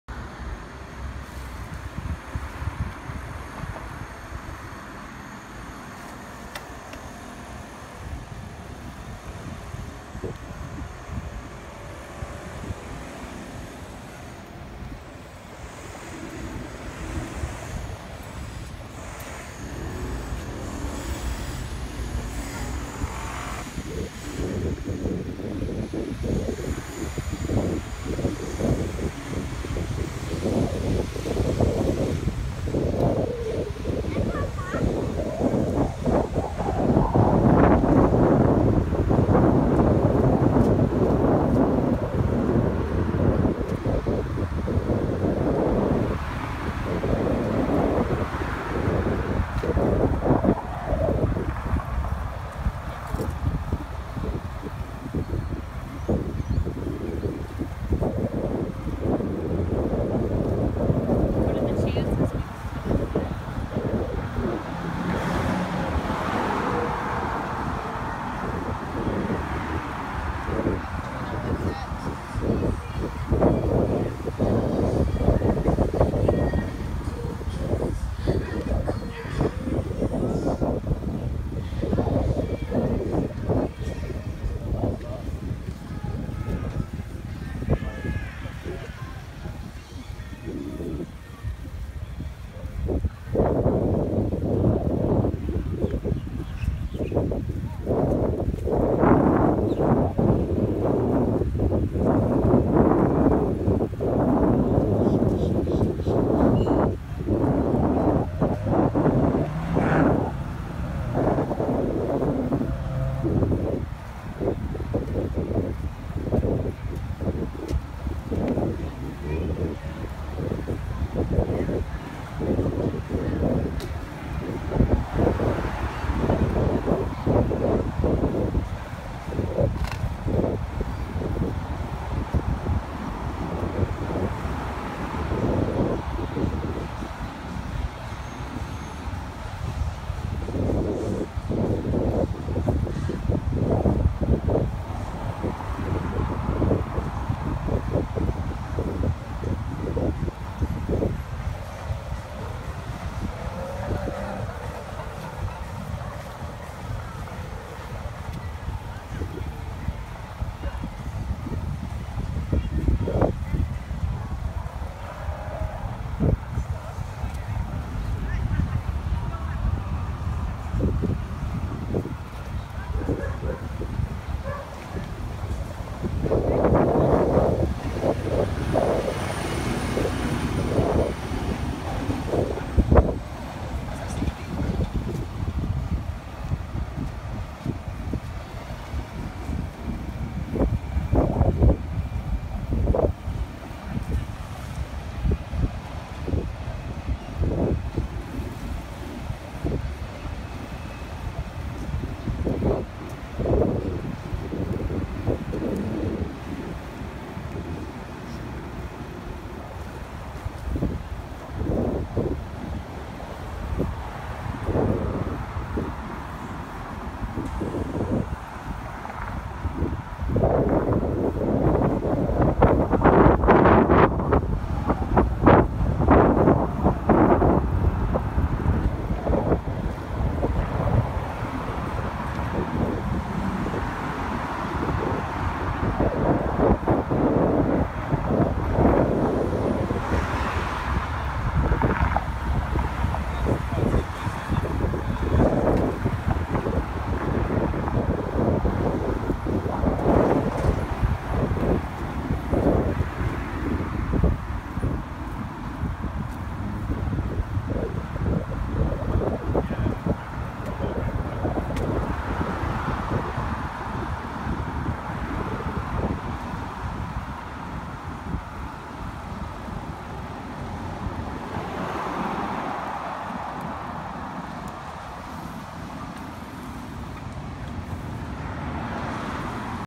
Cold day on Fort Lauderdale beach
This was what we call winter in SoFlo, somewhere around 60-65 degrees.